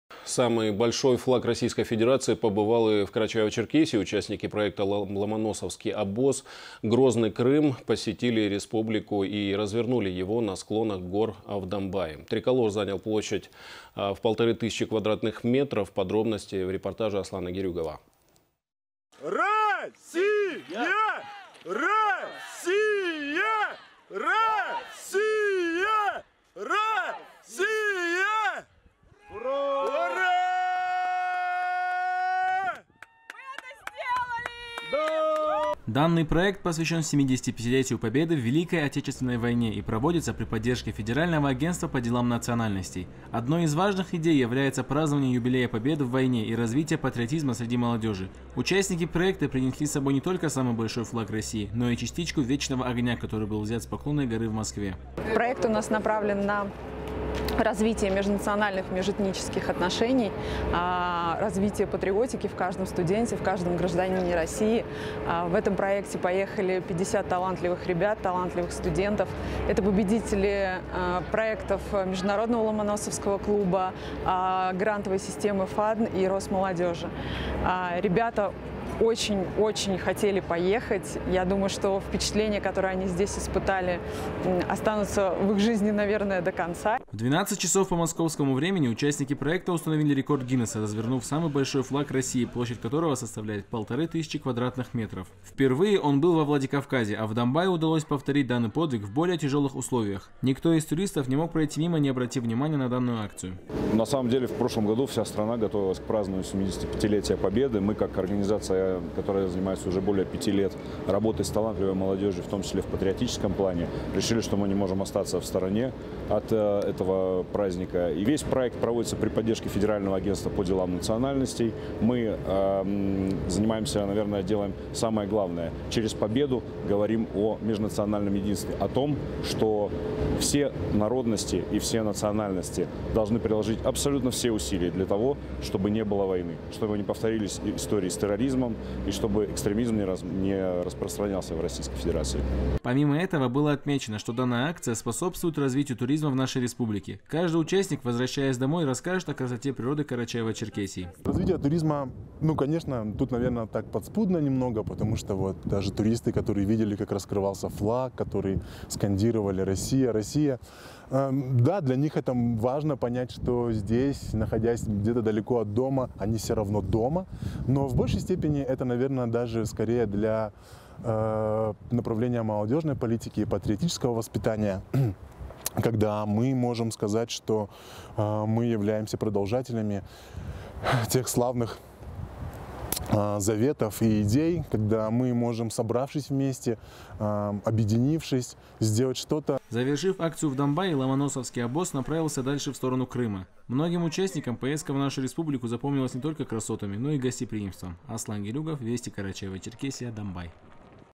Самый большой флаг Российской Федерации побывал и в Карачаево-Черкесии
Самый большой флаг Российской Федерации побывал и в Карачаево-Черкесии. Участники проекта «Ломоносовский обоз. Грозный-Крым» посетили республику и развернули его на склоне горы в Домбае